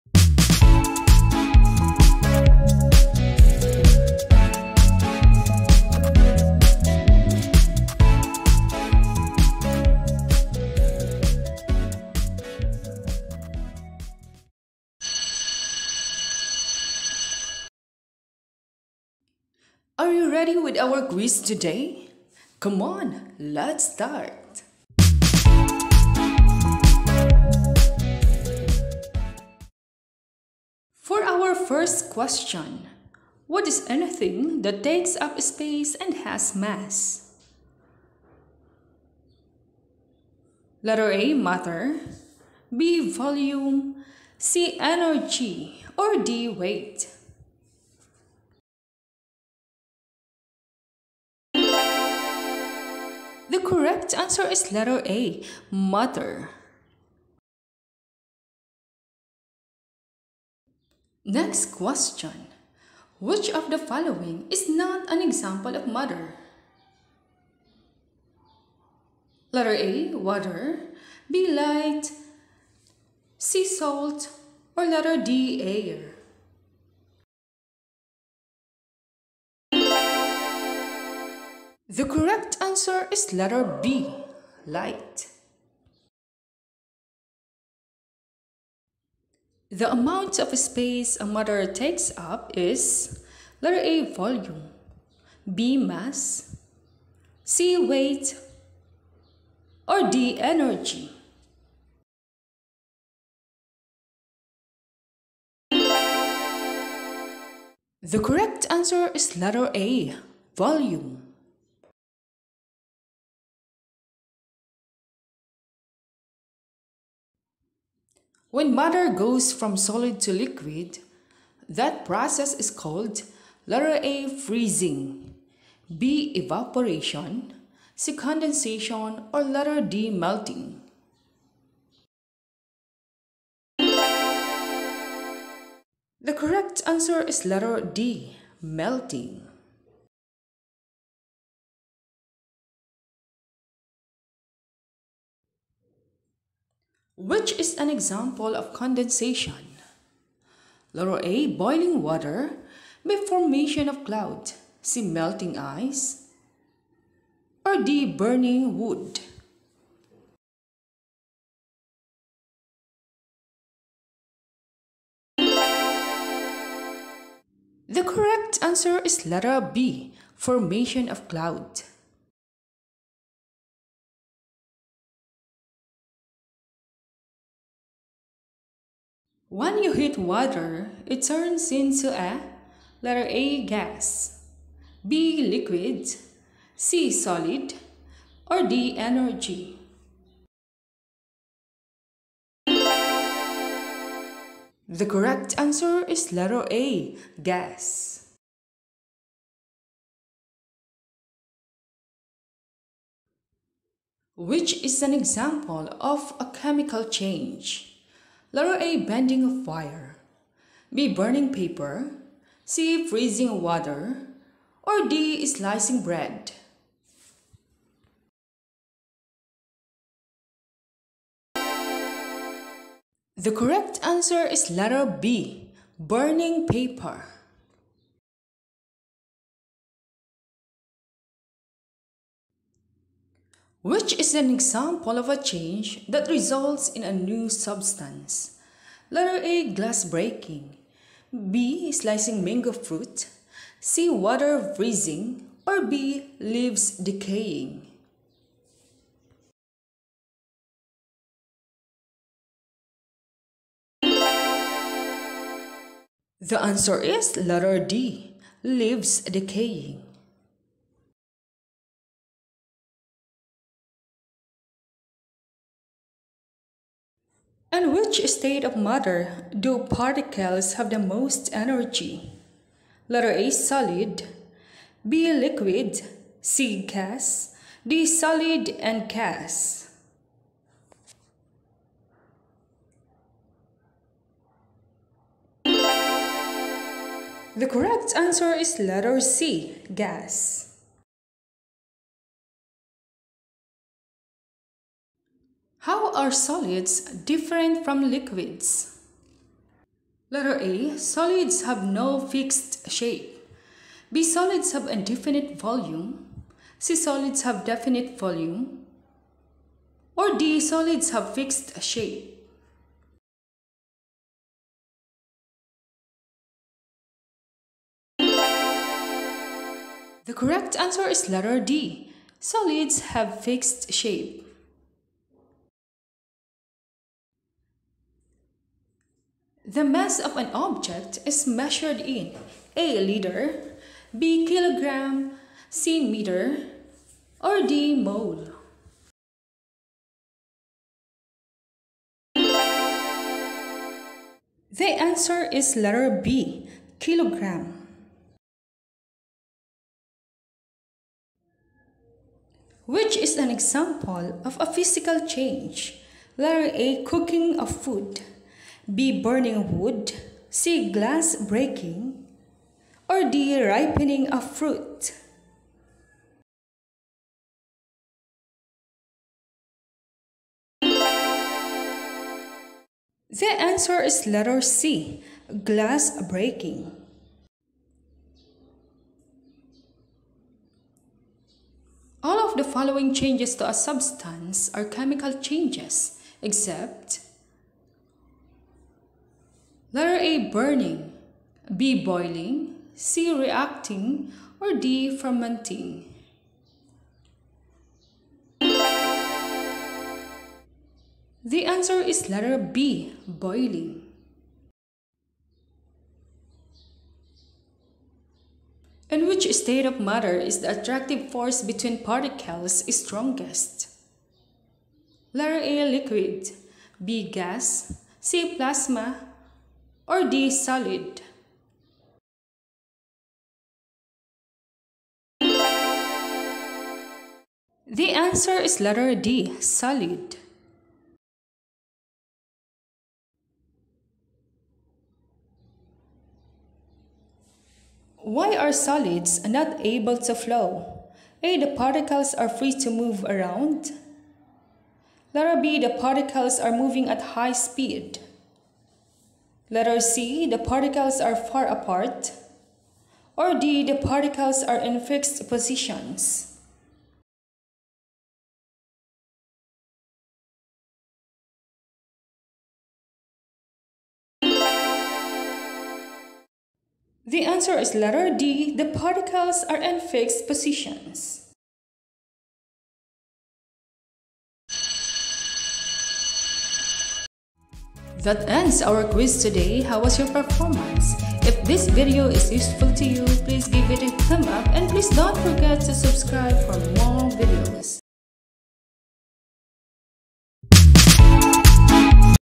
QUIZ | Matter | Part 1 | Entrance Exam Reviewer for incoming grade 7
Can you pass this elementary school test on Science?
this will help you recall your lesson on matter (elementary lessons)